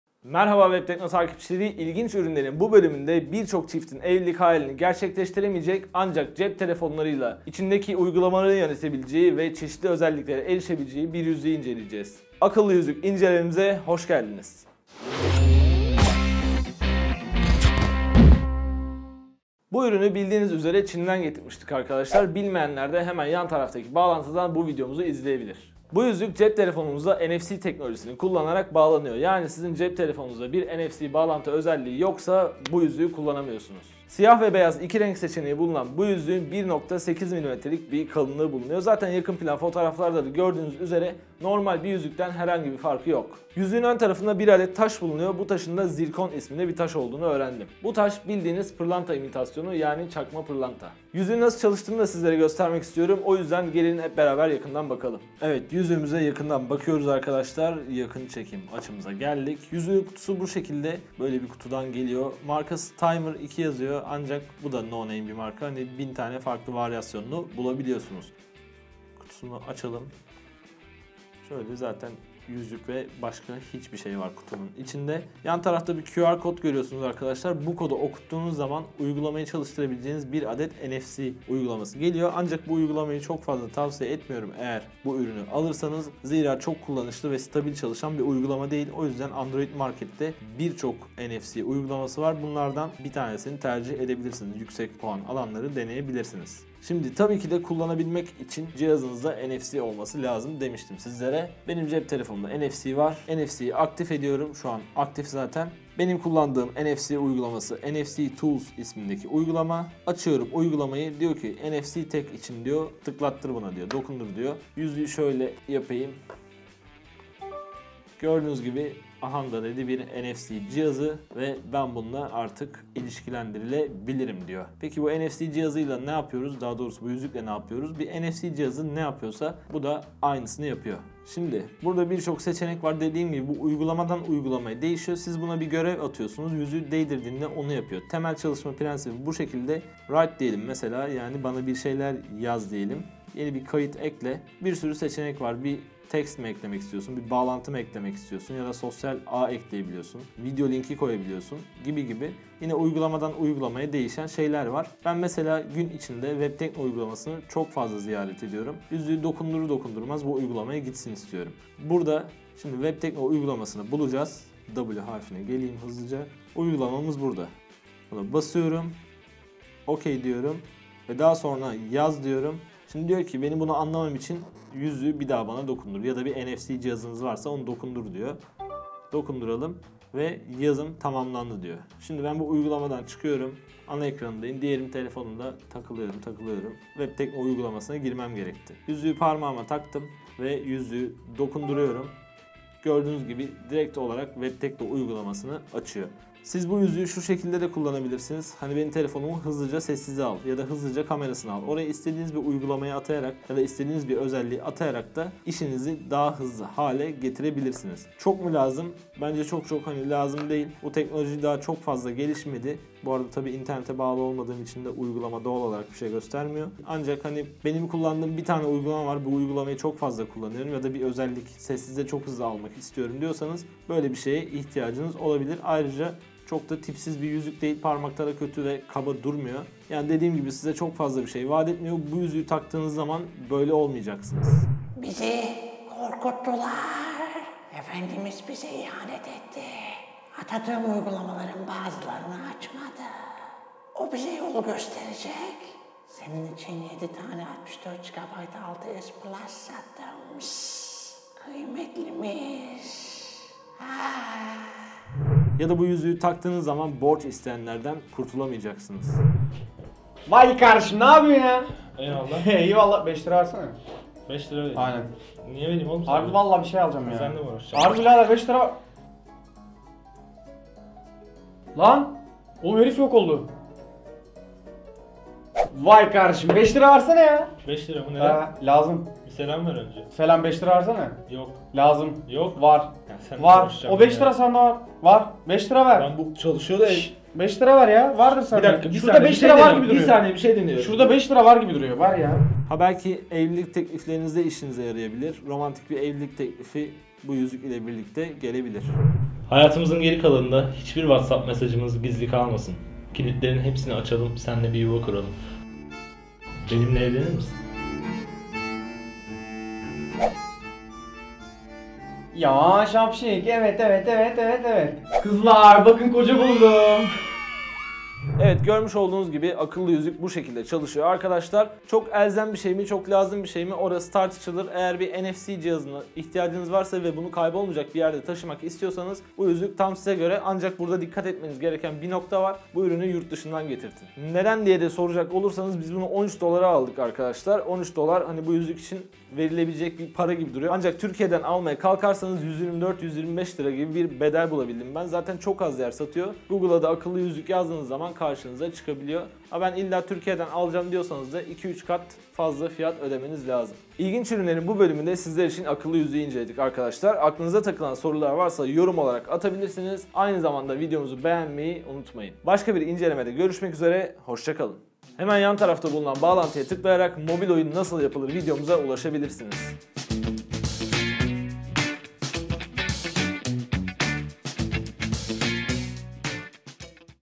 Akıllı Yüzük İncelemesi
İlginç ürünlerin yepyeni bir bölümüyle karşınızdayız. Bu videodaki konuğumuz NFC teknolojisi ile çalışan akıllı yüzük.